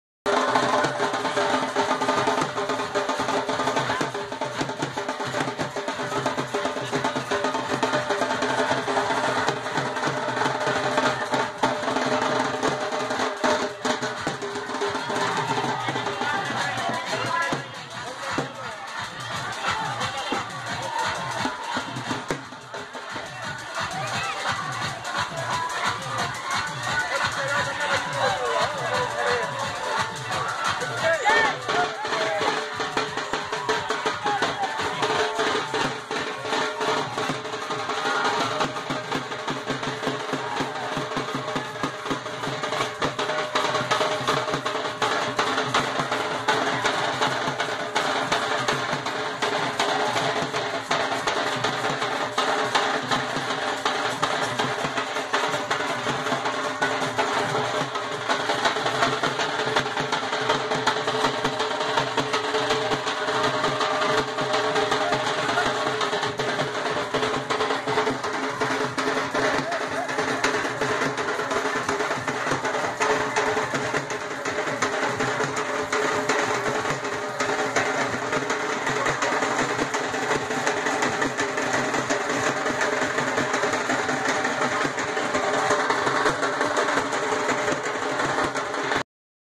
Madhai festival Mohad, Madhya Pradesh Year 2015
Madhai festival at my village Mohad, Madhya Pradesh, India, is a vibrant celebration that encapsulates the rich cultural heritage and traditional fervor of the region. Held annually, this festival is a significant occasion for the local communities, showcasing their deep-rooted connection with nature and spirituality. Madhai is marked by colorful processions, traditional dance performances, and religious rituals, all set against the backdrop of Madhya Pradesh's picturesque landscapes. Devotees come together to worship and seek blessings from deities, fostering a sense of unity and communal harmony. The festival serves as a poignant reminder of the cultural diversity that defines Madhya Pradesh, making Madhai a cherished and eagerly awaited event for locals and visitors alike.

Explore further my Tech, Memoir, Social & Thoughtful Life Experience Chronicle
On Telegram connect: @NextStruggleBot – Your direct line to